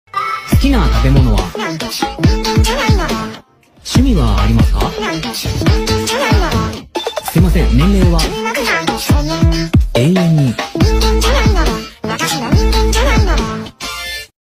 "Because I'm not human." | Markiplier Egos
It's been a while since I made Markiplier Ego content 😅

Characters, in order of appearance:
Eric Derekson
Googleplier Blue (Google)
King of the Squirrels
Googleplier Red (Google+)
Yandereplier
Googleplier Yellow (Google Play)
Googleplier Green (Google Maps)

[Apparently tags help boost videos in the algorithm, so just ignore these I guess? 😅]